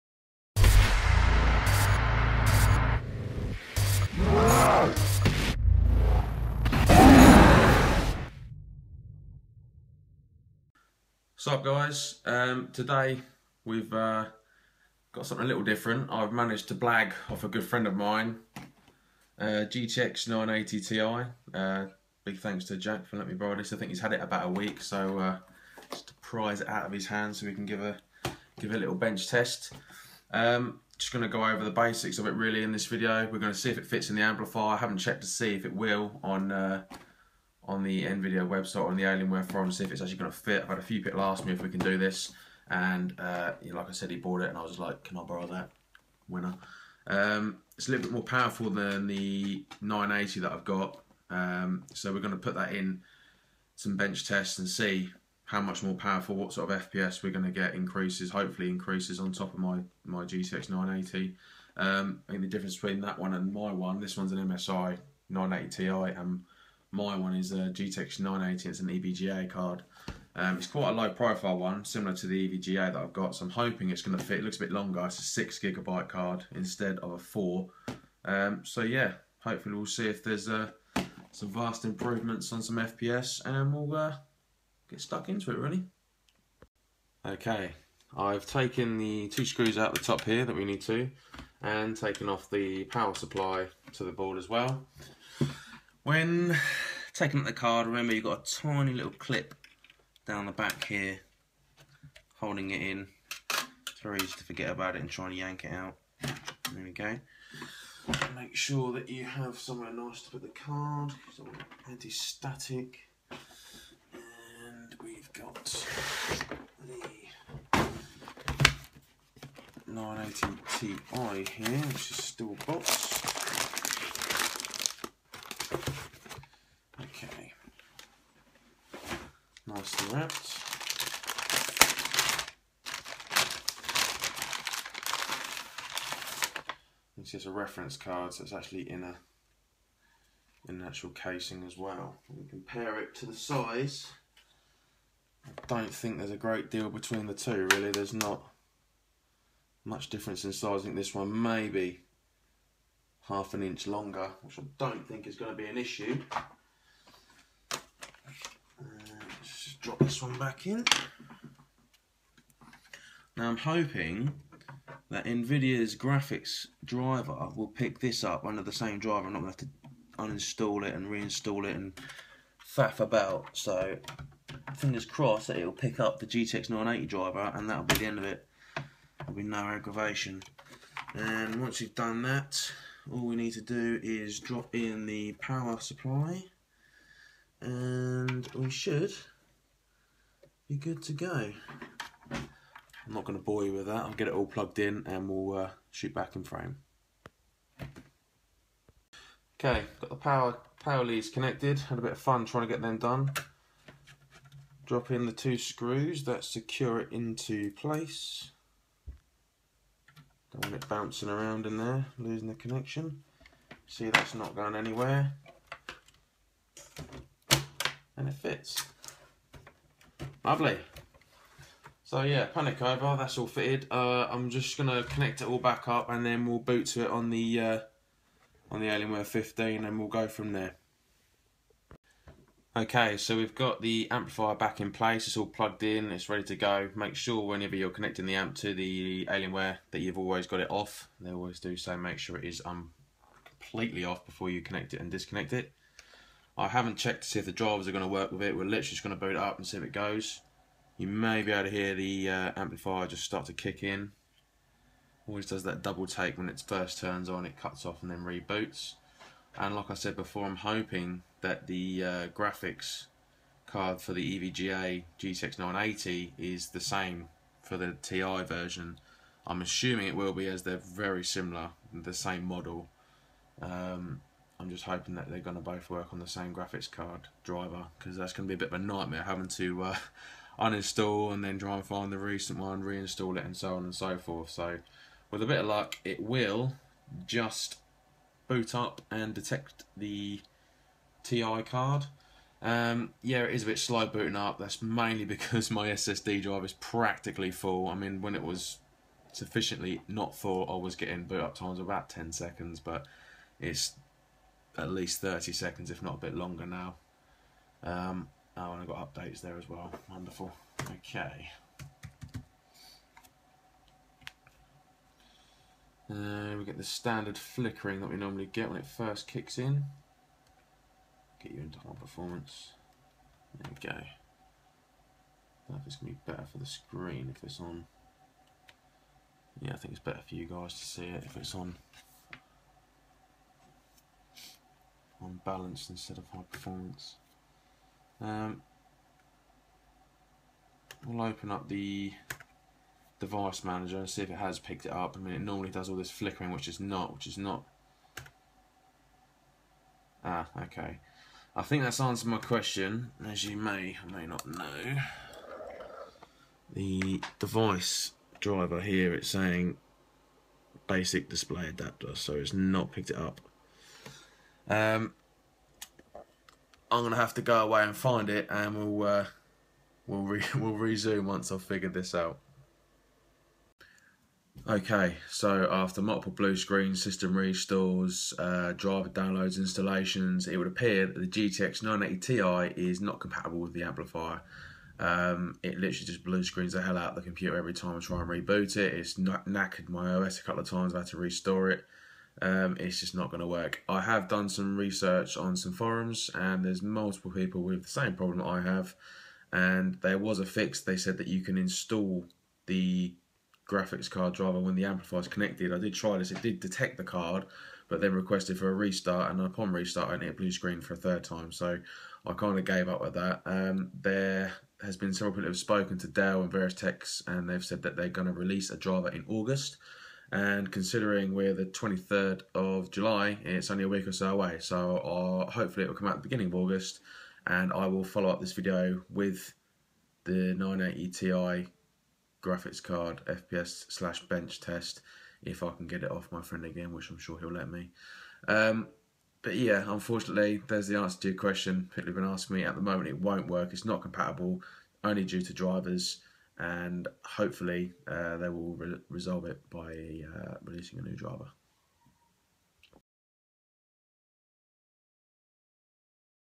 Alienware Graphics Amplifier - GTX 980 Ti Installation
Managed to get hold of an MSI GTX 980 Ti card and decided to see if it works in my Alienware Graphics Amplifier.